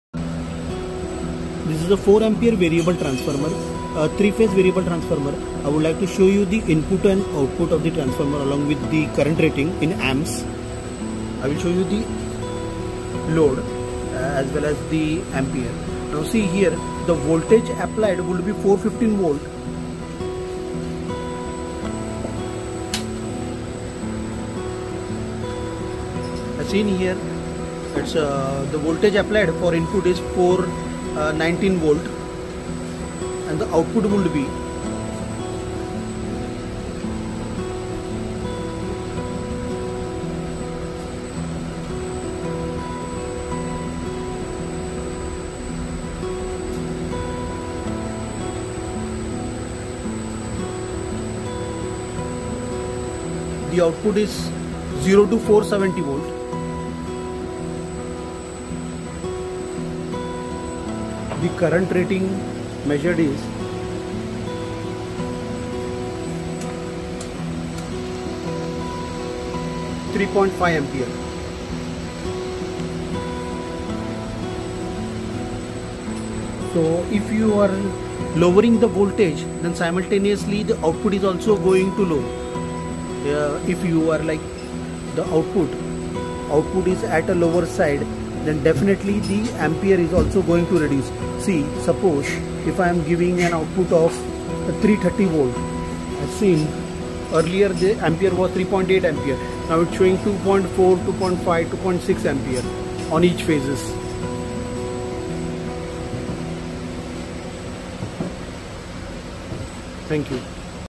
Variac 4Amps model no: 4F-3
Variac 4Ampere | Model no 4F-3 Variable Auto Transformer | Dimer | Regulator | Stabilizer.
   Variac 4Ampere.
*  Model no    : 4F-3.
*  INPUT       : 415V AC 50/60Hz..
*  OUTPUT      : 0-470V.
*  Phase       : Three Phase.
*  Type        : Open Type.
*   Dimensions in inches :- 9" x 7" x 21"